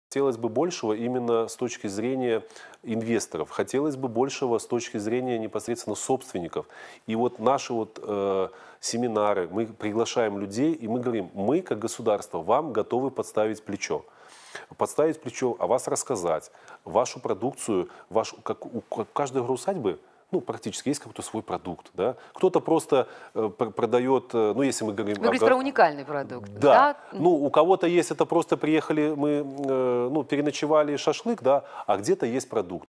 Чего не хватает туристической отрасли региона?
Рассказал начальник спорта и туризма Гомельского облисполкома Павел Старосветский.